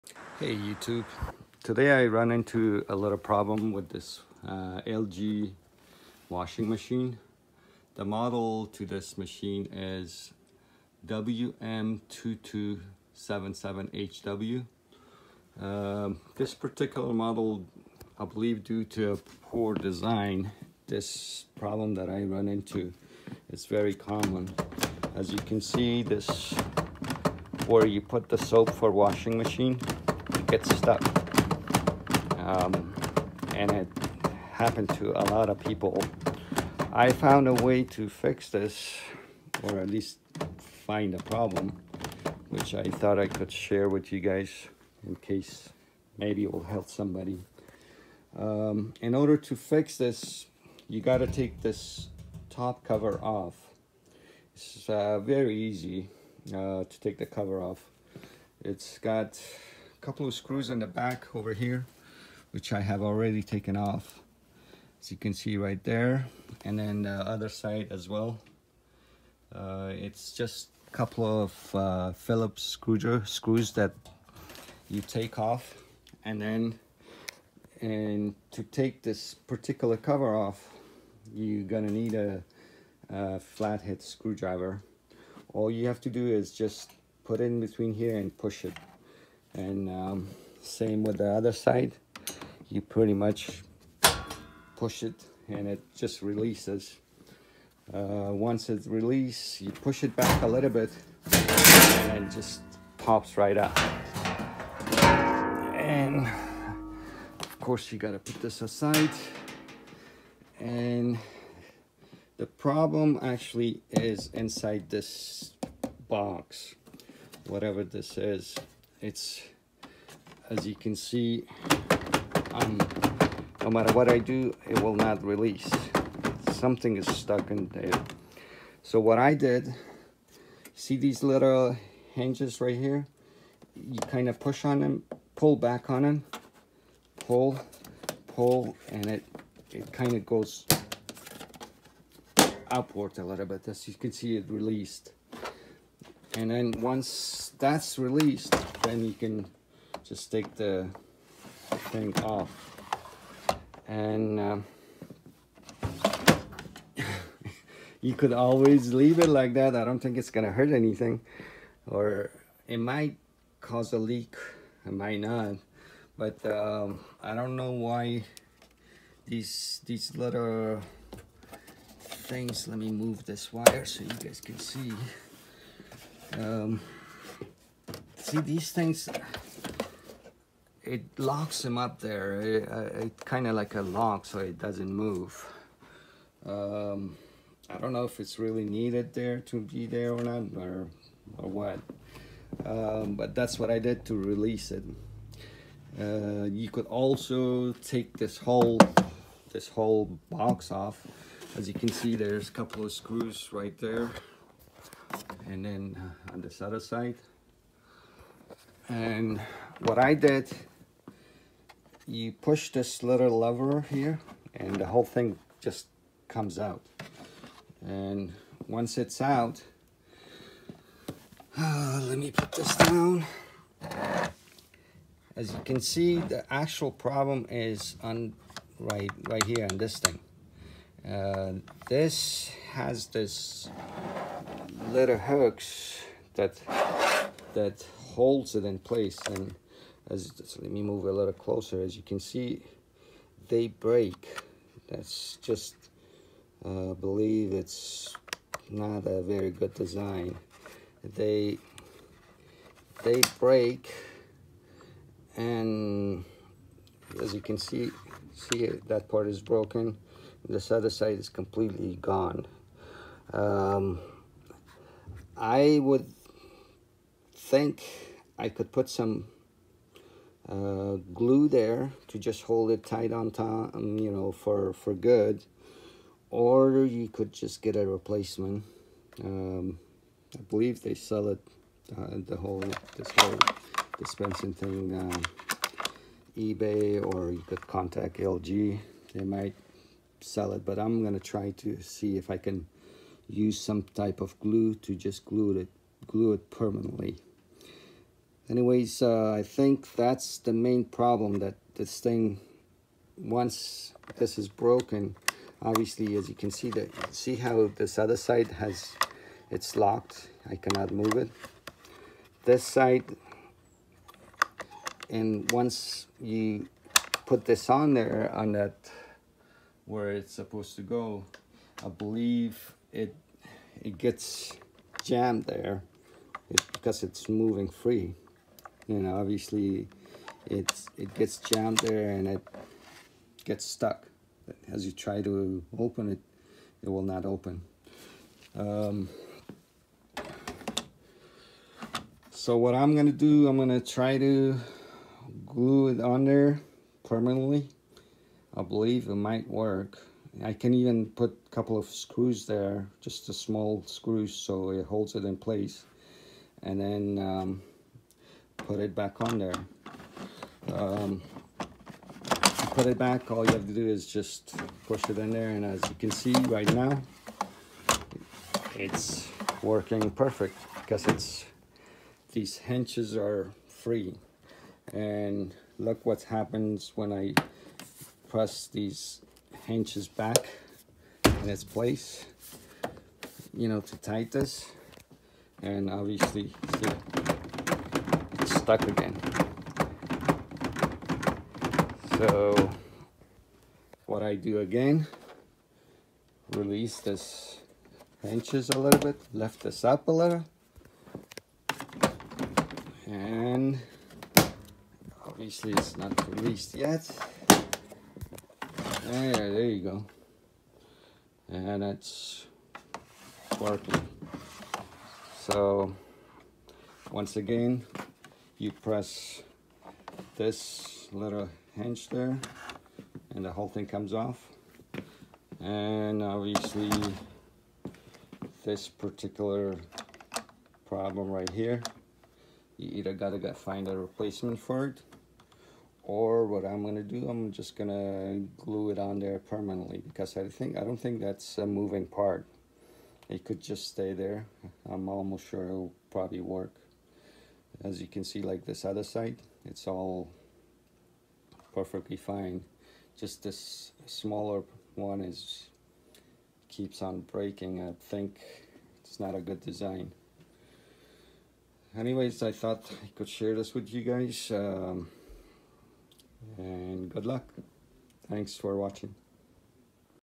How to fix Stuck LG soap dispenser on washing machine WM2277HW
LG Tromm washing machine stuck soap dispenser